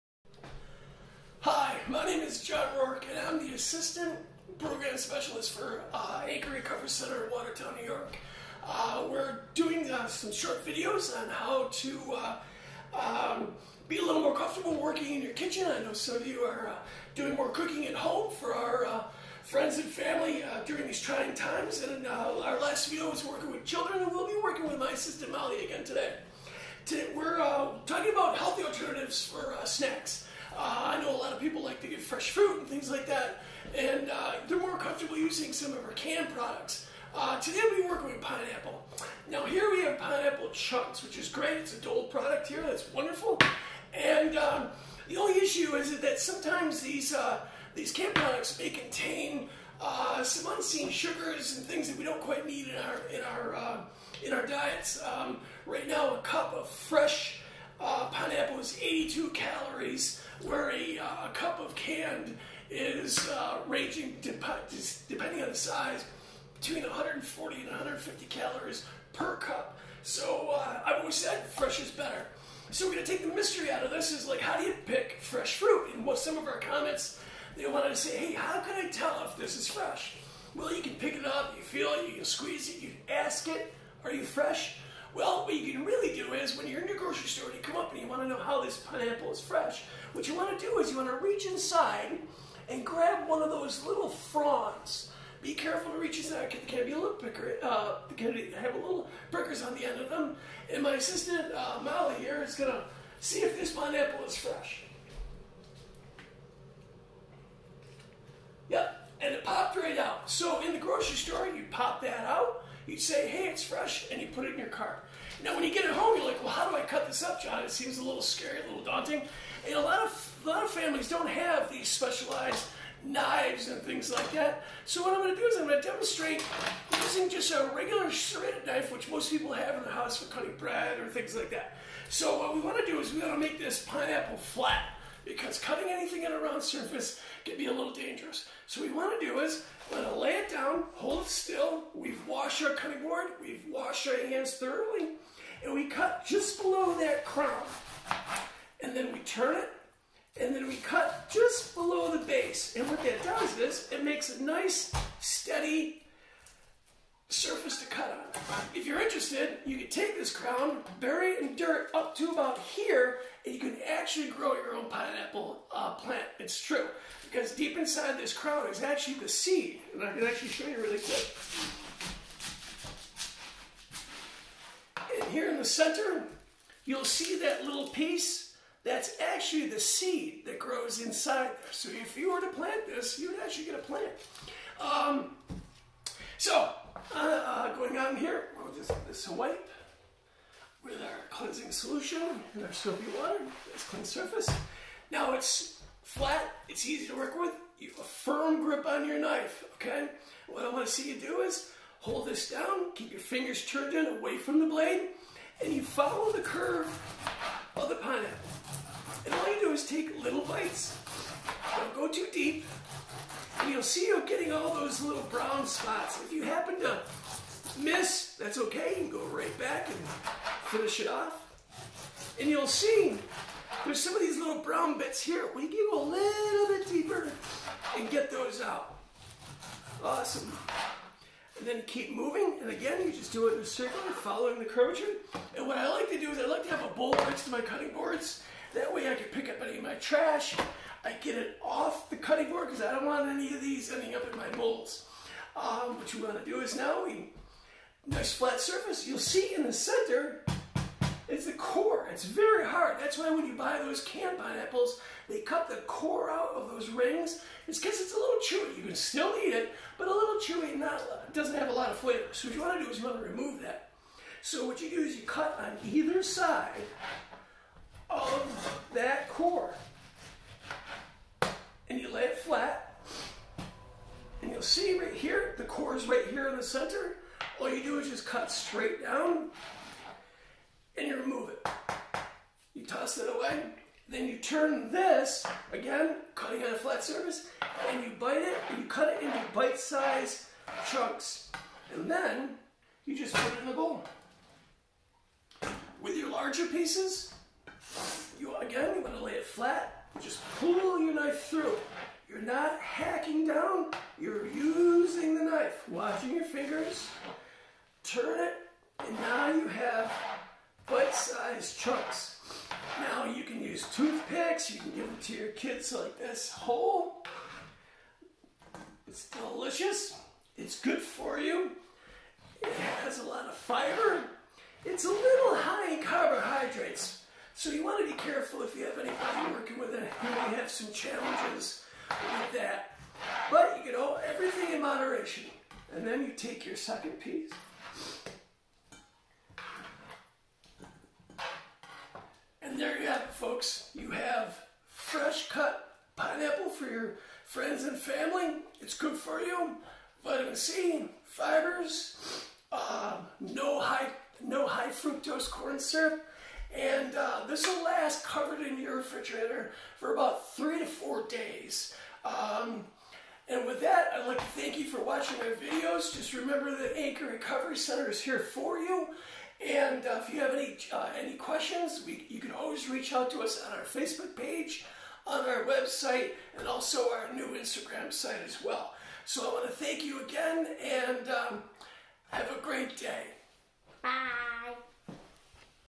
How-to Cut a Pineapple by Chef John Rork
Chef John Rork, Assistant Program Specialist at Anchor Recovery Center, walks you through a tutorial on how-to cut a pineapple. He also advises on the benefits of fresh pineapple versus canned.